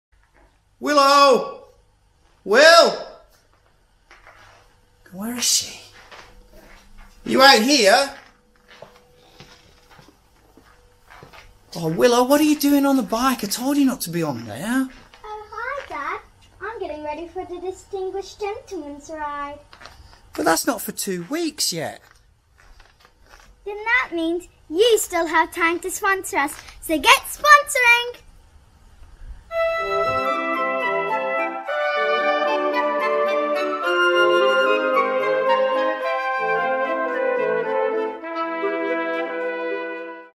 Distinguished Gentleman's Ride - Call for Sponsorship
Willow is dressed in her finest attire donning mustache and pipe, with the aim of collecting sponsorship for our upcoming ride in Newcastle NSW on Sunday 29th September 2019.